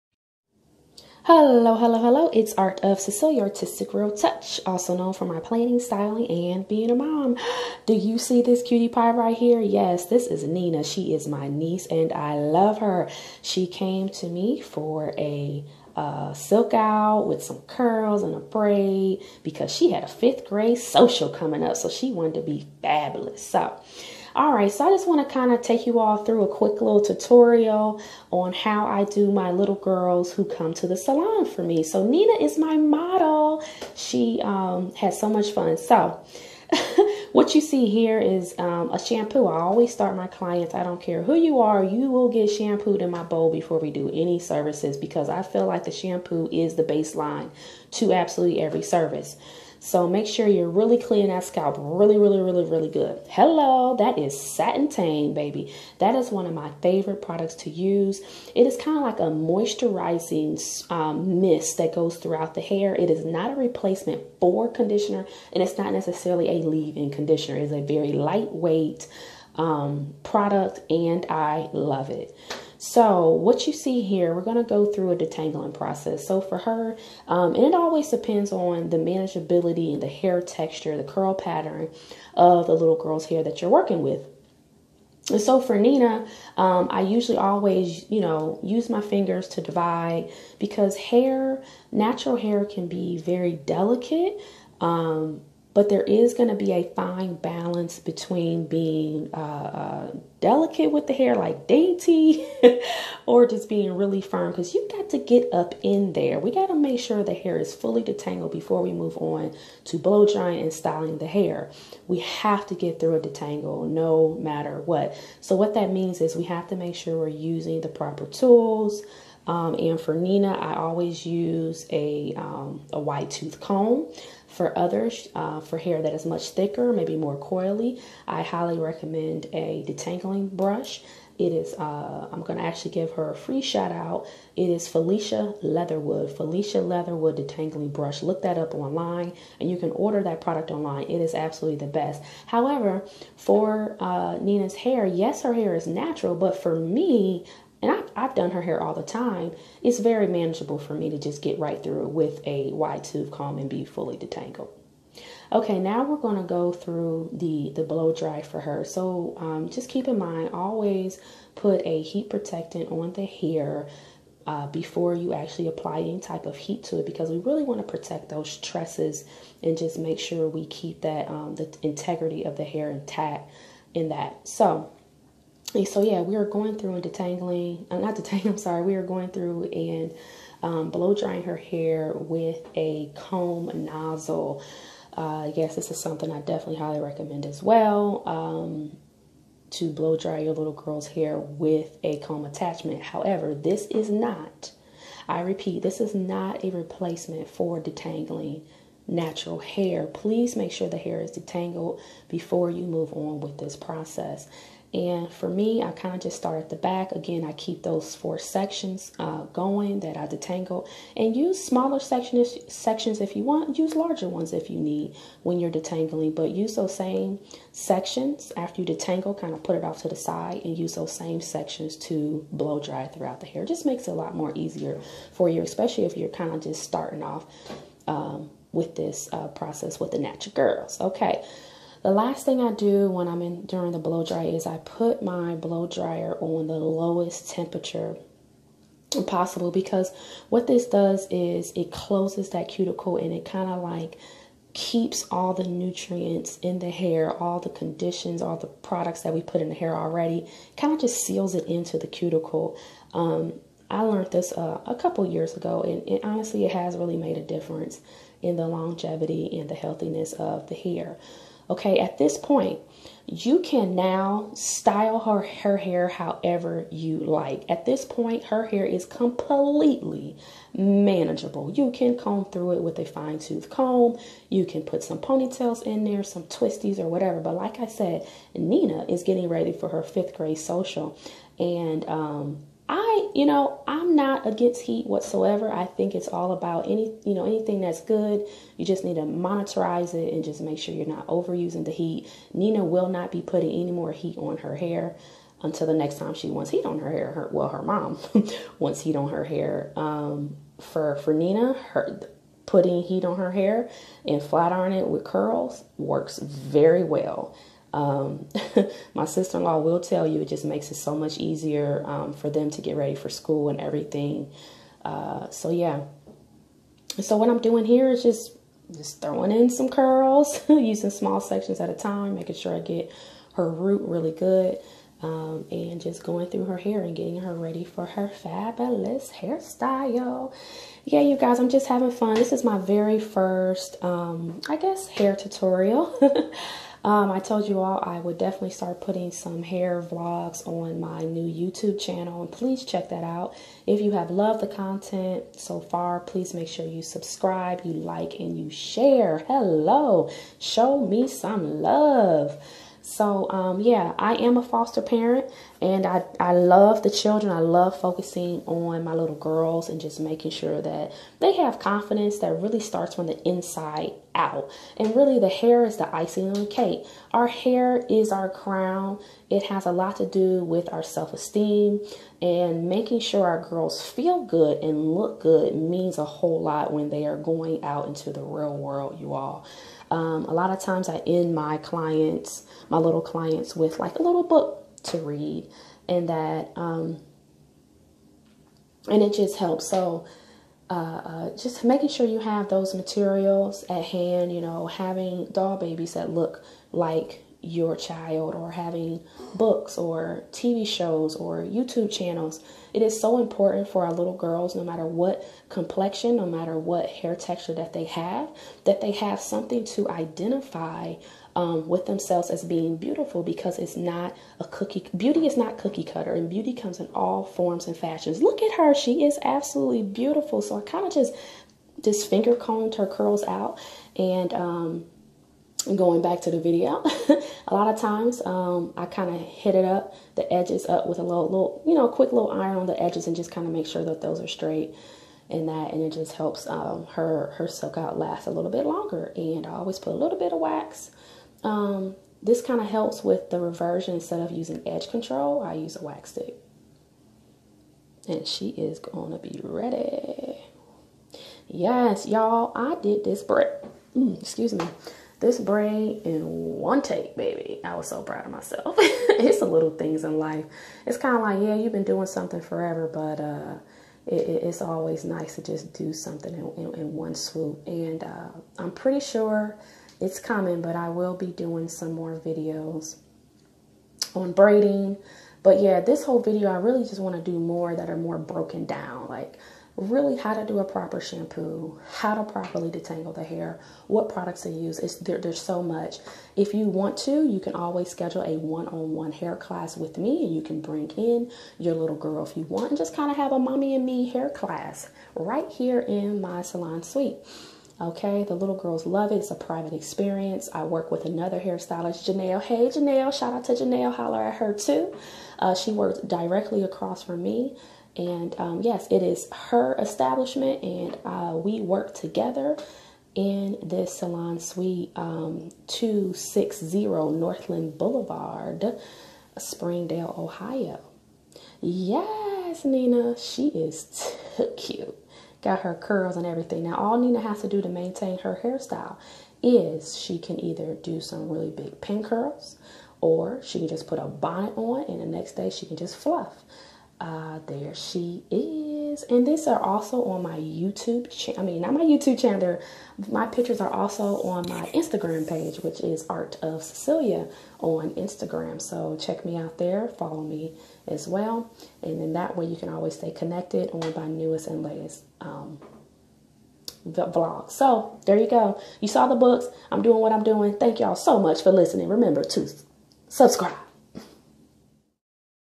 How to Style KIDS Natural hair from BEGINNING to END
My very first HAIR CARE VLOG!! Here I'm going to talk you through the process in which I shampooed, detangled, blow-dryed and styled my lil niece for her 5th Grade Social. Hope this vlog helps if you are a mom who wants to know more about handling your child's hair with textured. Thanks 😘💕😘

Book for FREE CONSULTATION
BOOK FOR 1on1 HAIR CLASS